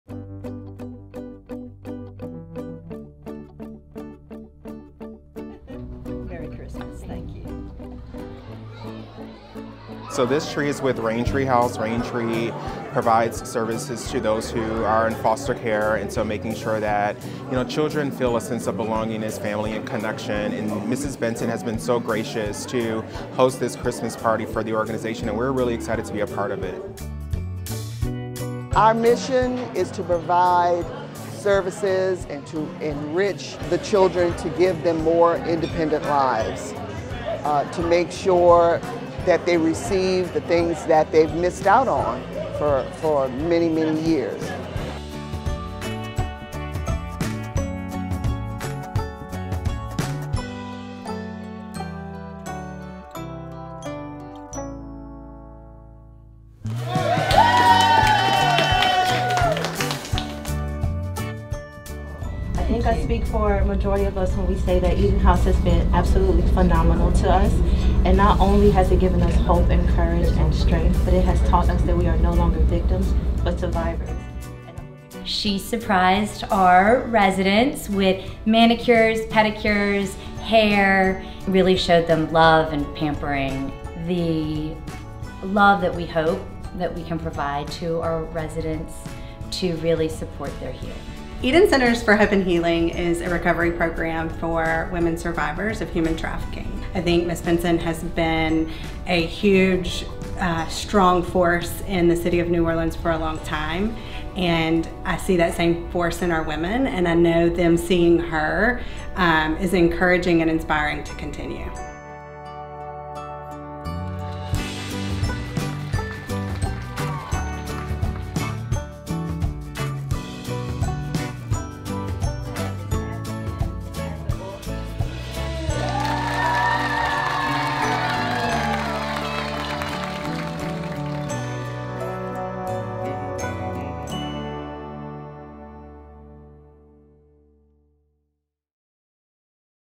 Recap: Gayle Benson's 2023 Season of Giving | New Orleans Saints
New Orleans Saints owner Gayle Benson held her annual Season of Giving by surprising local children and organizations with holiday parties, gifts, and donations during the 2023 holiday season.